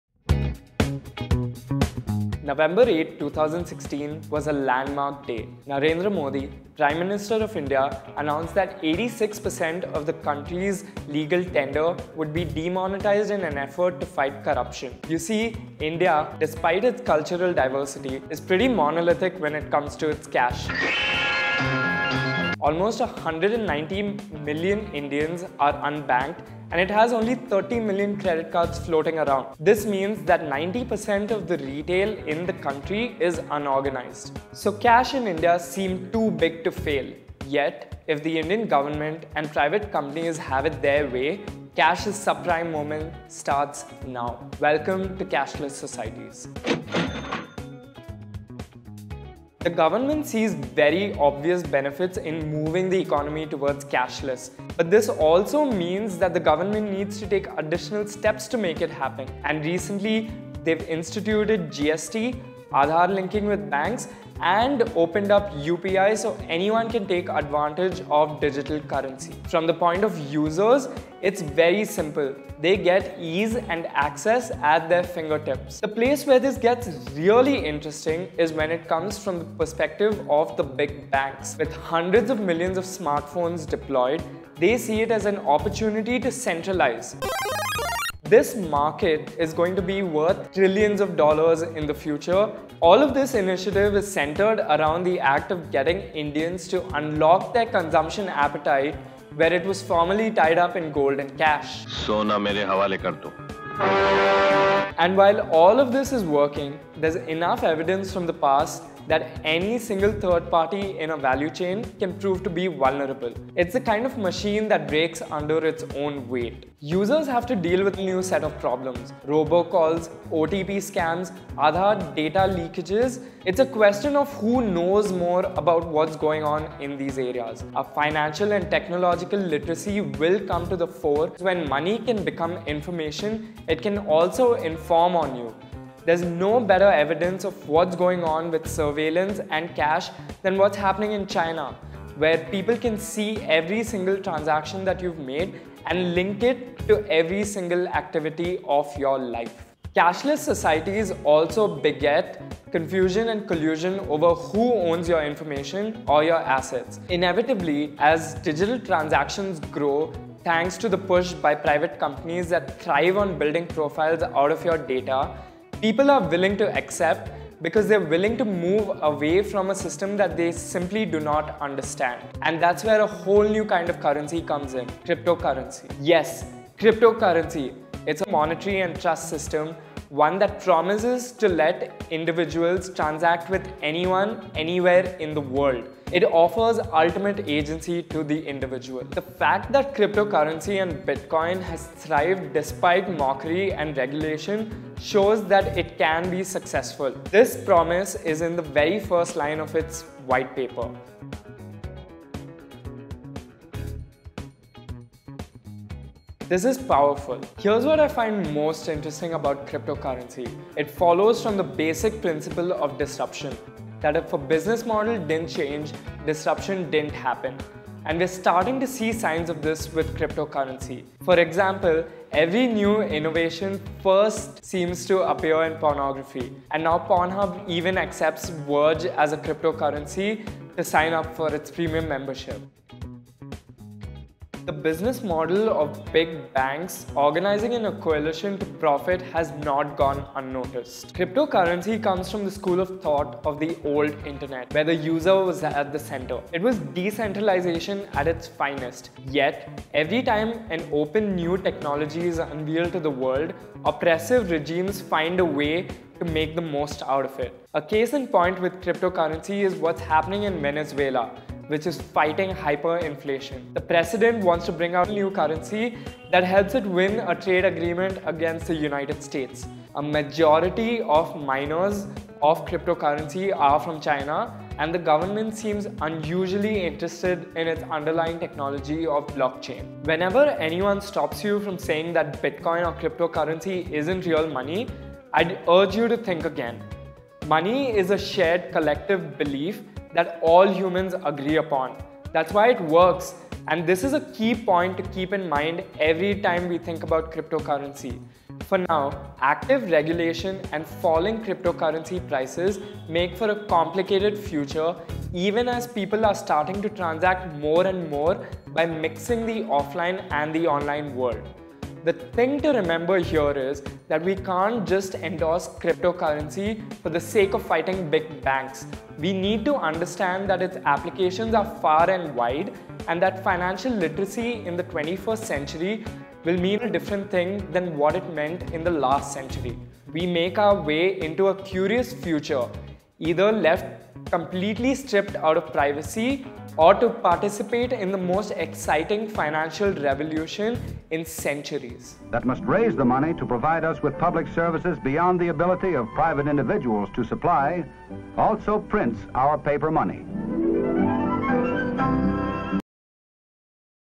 Cashless Societies | Things by Dentsu Webchutney
As money becomes information, financial literacy gets an all-new meaning.

Resources:

— Facebook tests the waters for a walled payments garden in India
— India’s largest digital wallet has been accused of handing over user data to the government
— The cashless society is a con – and big finance is behind it